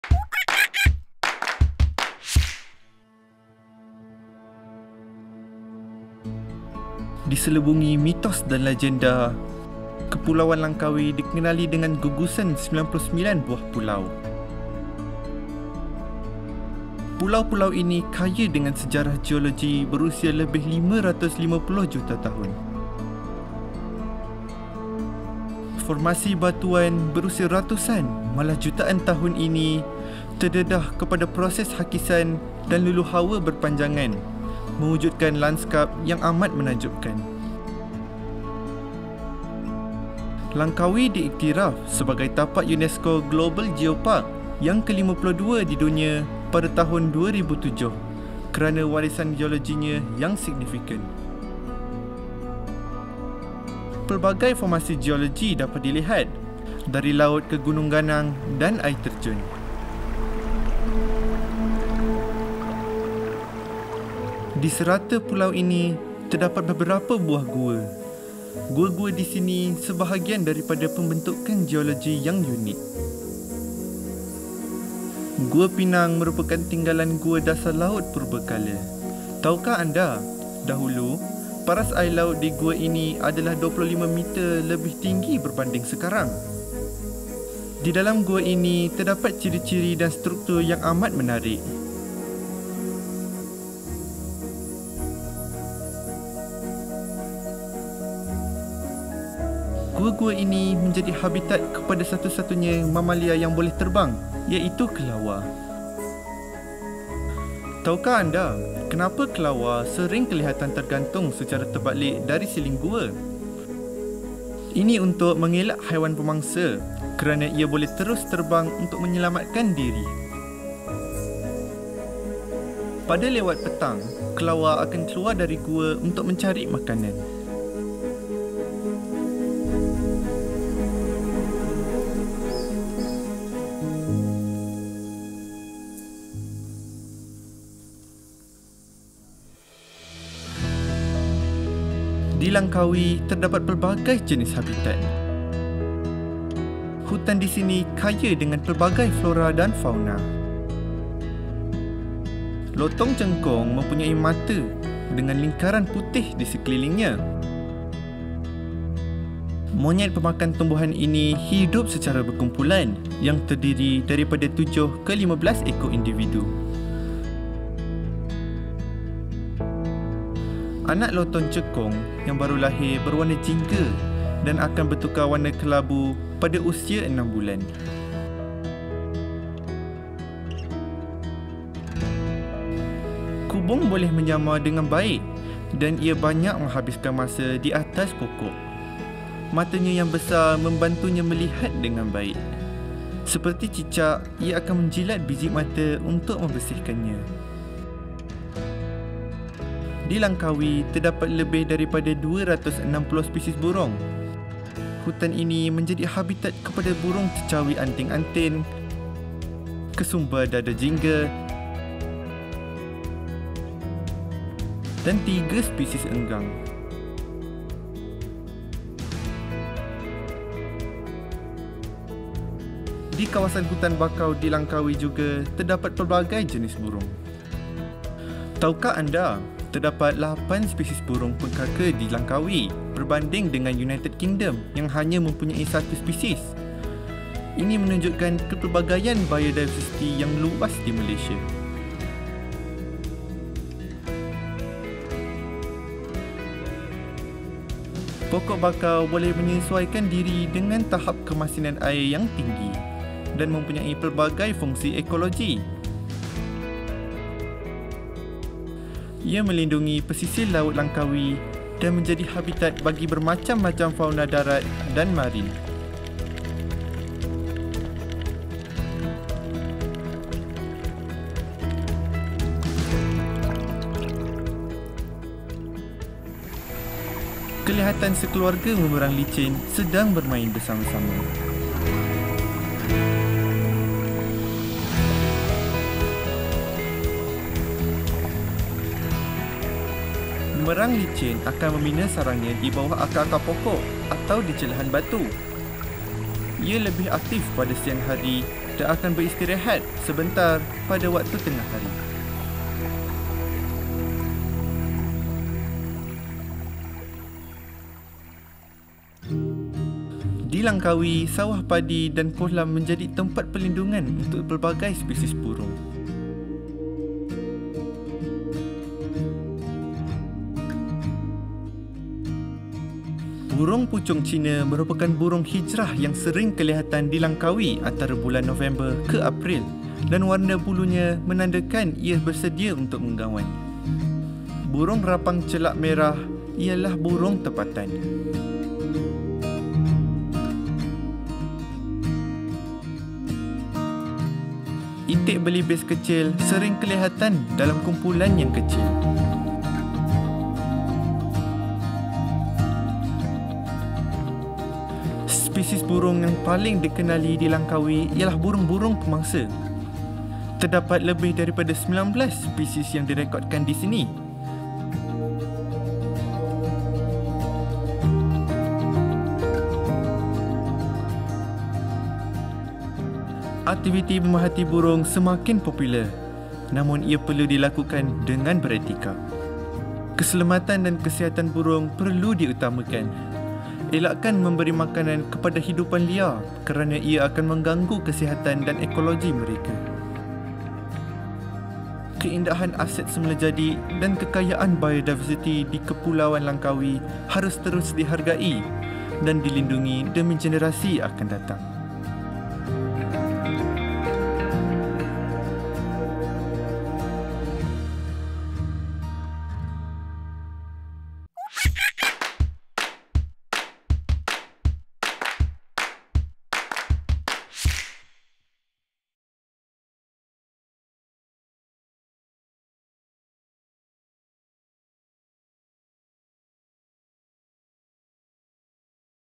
The Amazing Langkawi Island
Kaya dengan mitos dan lagenda, kepulauan Langkawi juga telah diiktiraf sebagai tapak Geopark UNESCO Global ke-52 di dunia kerana warisan geologinya yang amat signifikan. Saksikan episod kali ini untuk menjelajah keindahan Langkawi dan flora dan fauna yang hidup di sini!
Steeped in myths and legends, the Langkawi archipelago has also been endorsed as the 52nd Global UNESCO Geopark in 2007 for its significant geo-heritage. Tune in to this episode to explore Langkawi and the flora and fauna that live here!